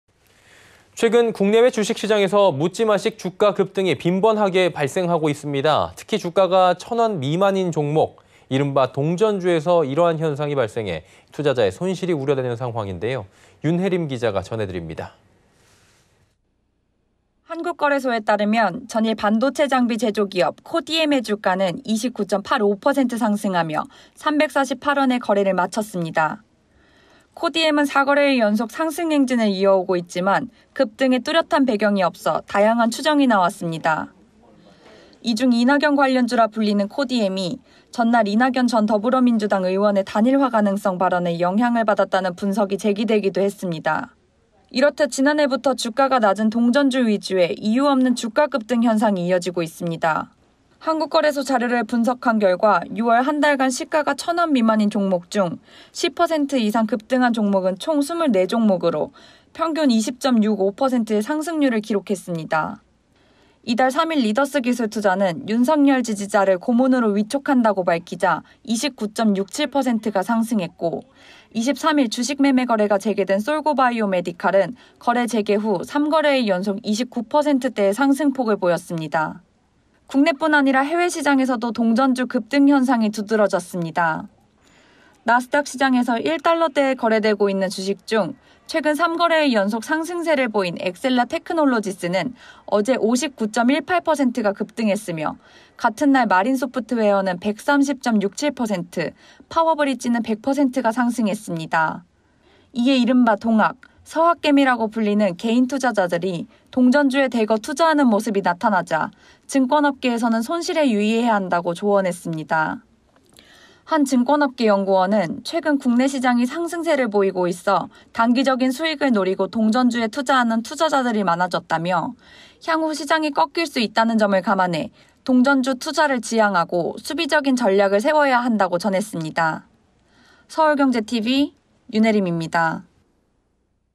‘동전주’ 이유 없는 급등…투자자 손실 주의
[앵커]
최근 국내외 주식시장에서 묻지마식 주가 급등이 빈번하게 발생하고 있습니다. 특히 주가가 1,000원 미만인 종목, 이른바 동전주에서 이런 현상이 발생해 투자자의 손실이 우려되는 상황입니다. 윤혜림 기잡니다.

[기자]
한국거래소에 따르면 전일(29일) 반도체 장비 제조기업 코디엠의 주가는 29.85% 상승하며 348원에 거래를 마쳤습니다.

코디엠은 4거래일 연속 상승 행진을 이어오고 있지만, 급등에 뚜렷한 배경이 없어 다양한 추정이 나왔습니다.

이중 이낙연 관련주라 불리는 코디엠이 전날 이낙연 전 더불어민주당 의원의 단일화 가능성 발언에 영향을 받았다는 분석이 제기되기도 했습니다.

이렇듯 지난해부터 주가가 낮은 동전주 위주의 이유 없는 주가 급등 현상이 이어지고 있습니다.

한국거래소 자료를 분석한 결과 6월 한 달간 시가가 1,000원 미만인 종목 중 10% 이상 급등한 종목은 총 24종목으로 평균 20.65%의 상승률을 기록했습니다.

이달 3일 리더스기술투자는 윤석열 지지자를 고문으로 위촉한다고 밝히자 29.67%가 상승했고, 23일 주식 매매거래가 재개된 솔고바이오메디칼은 재개 후 3거래일 연속 29%대의 상승폭을 보였습니다.

국내뿐 아니라 해외시장에서도 동전주 급등 현상이 두드러졌습니다.

나스닥 시장에서 1달러 대에 거래되고 있는 주식 중 최근 3거래일 연속 상승세를 보인 엑셀라테크놀로지스는 어제(현지시간 29일) 59.18%가 급등했으며, 같은 날 마린 소프트웨어는 130.67%, 파워브릿지는 100%가 상승했습니다.

이에 이른바 동학·서학개미라고 불리는 개인투자자들이 동전주에 대거 투자하는 모습이 나타나자 증권업계에서는 손실에 유의해야 한다고 조언했습니다.

한 증권업계 연구원은 “최근 국내 시장이 상승세를 보이고 있어 단기적인 수익을 노리고 동전주에 투자하는 투자자들이 많아졌다”며 “향후 시장이 꺾일 수 있다는 점을 감안해 동전주 투자를 지양하고 수비적인 전략을 세워야 한다”고 전했습니다.